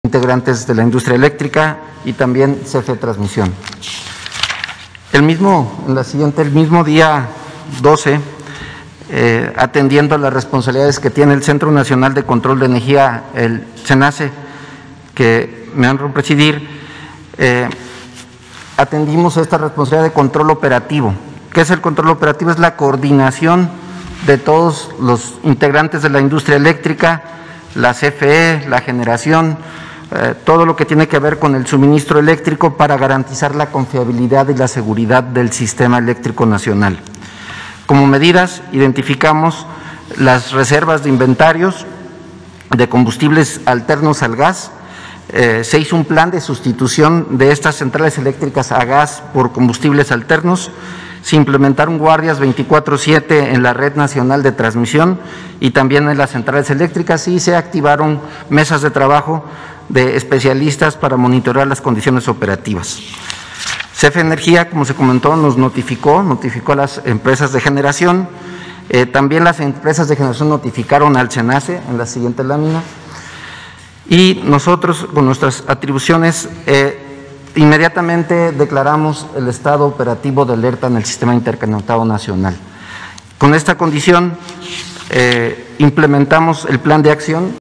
Carlos Meléndez describe las acciones para garantizar el suministro eléctrico ante frente frío 35
El director del Centro Nacional de Control de Energía (Cenace), Carlos Meléndez, describe las acciones para garantizar el suministro eléctrico ante el frente frío número 35; que decretó la alerta de posible desabasto de gas desde el viernes 12 de febrero para México